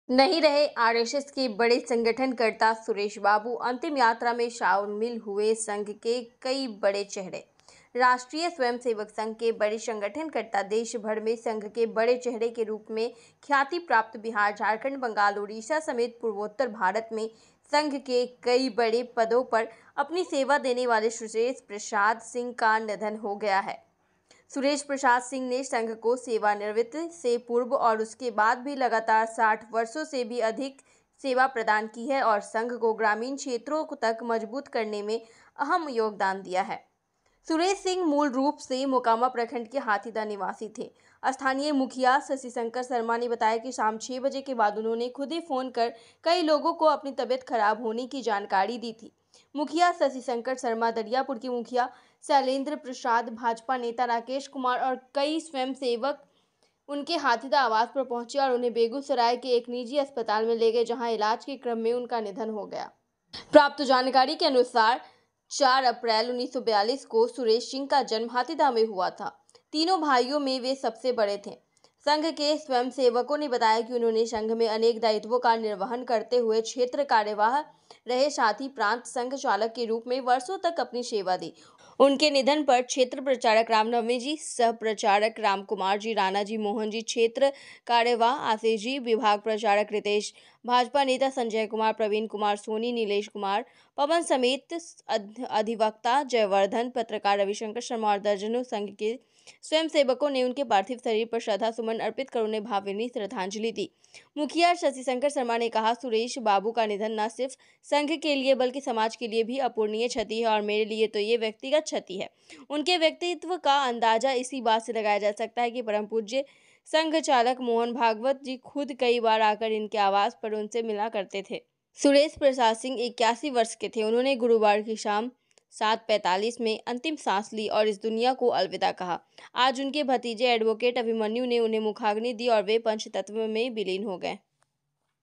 नहीं रहे आर एस एस के बड़े संगठन कर्ता सुरेश बाबू,अंतिम यात्रा में शामिल हुये संघ के बड़े चेहरे
नहीं रहे आर एस एस के बड़े संगठन कर्ता सुरेश बाबू,अंतिम यात्रा में शामिल हुये संघ के बड़े चेहरे l Swaraj Bharat Live

राष्ट्रीय स्वयं सेवक संघ के बड़े संगठन कर्ता, देश भर में संघ के बड़े चेहरे के रूप में ख्यातिप्राप्त, बिहार झारखण्ड, बंगाल उड़ीसा समेत पुर्वोत्तर भारत में में संघ के कई बड़े पदों पर अपनी सेवा देने वाले सुरेश प्रसाद सिंह का निधन हो गया!
श्री सिंह ने संघ को सेवा निवृति से पूर्व और उसके बाद भी लगातार 60 वर्षों से भी अधिक सेवाएं दी और संघ को ग्रामीण क्षेत्रों तक मजबूत करने में अहम योगदान दिया!
श्री सुरेश सिंह मूल रूप से मोकामा प्रखंड के हाथीदह निवासी थे!
स्थानीय मुखिया शशि शंकर शर्मा ने बताया की शाम 6 बजे के बाद उन्होंने खुद ही फोन कर कई लोगों को अपनी तबियत खराब होने की जानकारी दी!
मुखिया शशि शंकर शर्मा, दरियापुर के मुखिया शैलेन्द्र प्रसाद, भाजपा नेता राकेश कुमार और कई स्वयं सेवक उनके हाथीदह आवास पर पहुँचे और उन्हें बेगूसराय के एक निजी अस्पताल ले गये जहाँ ईलाज के क्रम में उनका निधन हो गया!
प्राप्त जानकारी के अनुसार 04/04/1942 को श्री सिंह का जन्म हथिदह में हुआ, तीन भाइयों में वे सबसे बड़े थे!
संघ के स्वयं सेवकों ने बताया की उन्होंने संघ में अनेक दायित्वों का निर्वहन करते हुये, क्षेत्र कार्यवाह रहे साथ ही प्रांत संघ चालक के रूप में वर्षों तक अपनी सेवा दी!
उनके निधन पर क्षेत्र प्रचारक राम नवमी जी, सह प्रचारक राम कुमार जी,राणा जी, मोहन जी, क्षेत्र कार्यवाह आशीष जी, विभाग प्रचारक रितेश जी, भाजपा नेता संजय कुमार, प्रवीण कुमार सोनी, निलेश कुमार पवन समेत अधिवक्ता जयवर्धन जी, पत्रकार रवि शंकर शर्मा और दर्जनों संघ के स्वयं सेवकों ने उनके पार्थिव शरीर पर श्रद्धा सुमन अर्पित कर उन्हें भावभीनी श्रद्धांजलि दी!
मुखिया शशि शंकर शर्मा ने कहा
सुरेश बाबू का निधन ना सिर्फ संघ के लिये बल्कि समाज के लिये भी अपूर्णीय क्षति है और मेरे लिये तो ये व्यक्तिगत क्षति है! उनके व्यक्तित्व का अंदाजा इसी बात से लगाया जा सकता है की परम् पूज्य सर संघ चालक मोहन भागवत जी खुद कई बार आकर उनके आवास पर उनसे मिला करते थे!
श्री सिंह 81 वर्ष के थे, उन्होंने गुरुवार की शाम 7:45 बजे अंतिम सांस ली और इस दुनियाँ को अलविदा कहा!
आज उनके भतीजे एडवोकेट अभिमन्यु ने उन्हें मुखअग्नि दी और वे पंचतत्व में विलीन हो गये!